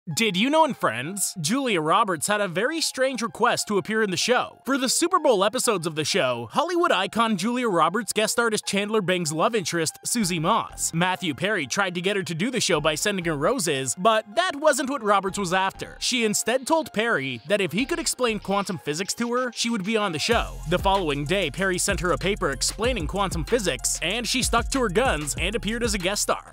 Did You Know In FRIENDS…
Julia Roberts had a very strange request to appear in the show! For the superbowl episodes of the show, Hollywood icon Julia Roberts guest starred as Chandler Bing’s love interest, Susie Moss. Matthew Perry tried to get her to do the show by sending her roses, but that wasn’t what Roberts was after. She instead told Perry that if he could explain quantum physics to her she would be on the show. The following day Perry sent her a paper explaining quantum physics and she stuck to her guns and appeared as a guest star.